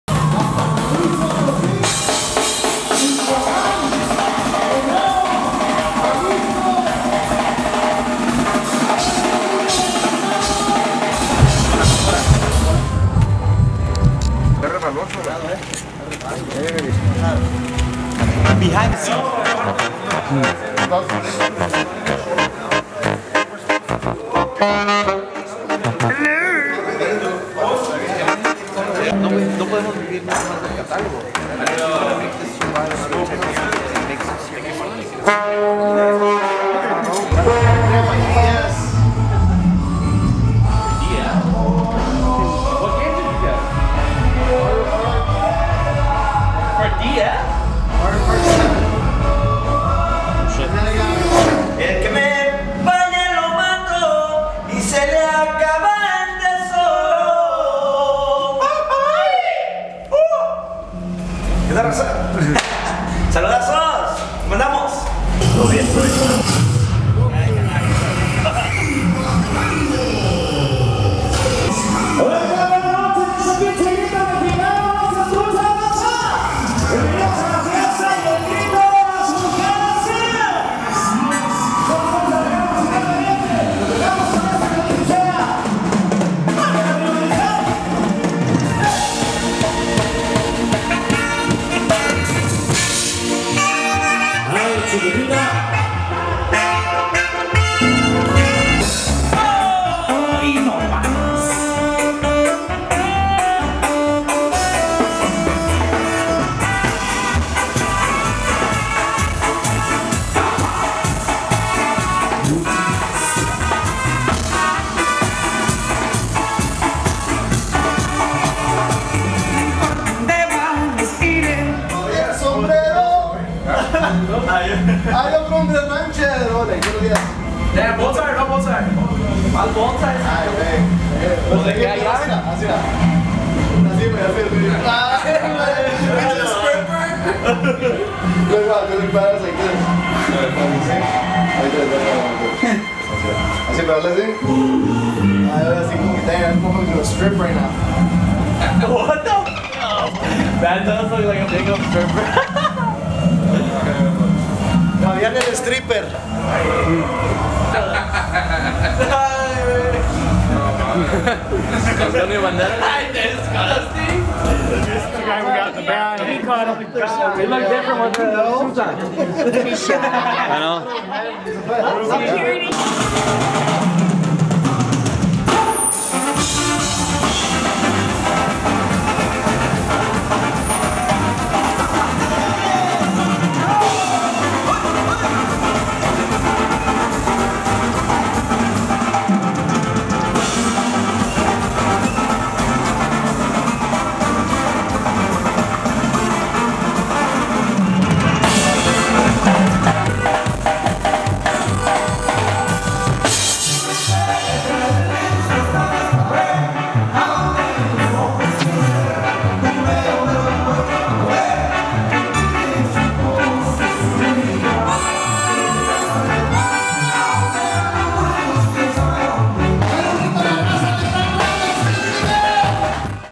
Montez De Durango Tour Arriba La Sierra Vlog 1.
Tour Arriba La Sierra 2018!
Grand Rapids, MI. 1/12/18
Indianapolis, IN. 1/13/18
Columbus, OH. 1/14/18